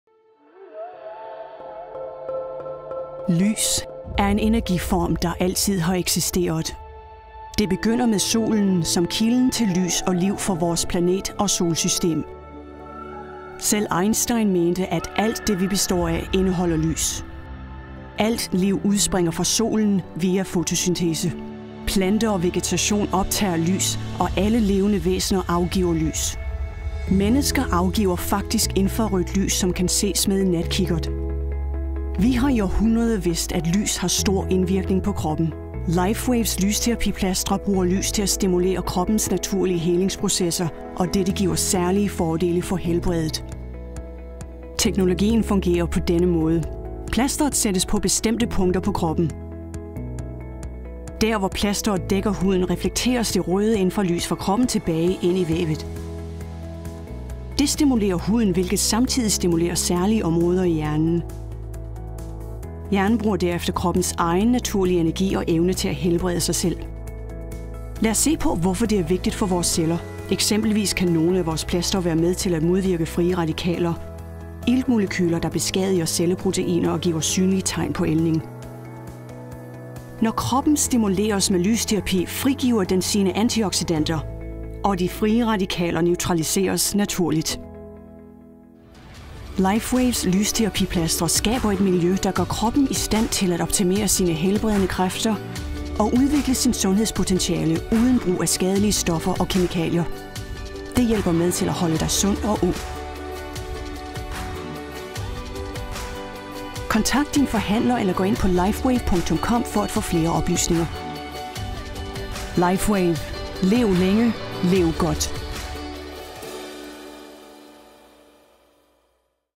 Hvad er lysterapi ? - også kaldet fototerapi? Dansk
Se videoen og lær om lysterapi og om hvordan Lifewave plastrene virker.
Plastrene er non-transmerdale og virker kun vha lysterapi og intet kommer ind i kroppen.

Lifewave independent distributor ID 771372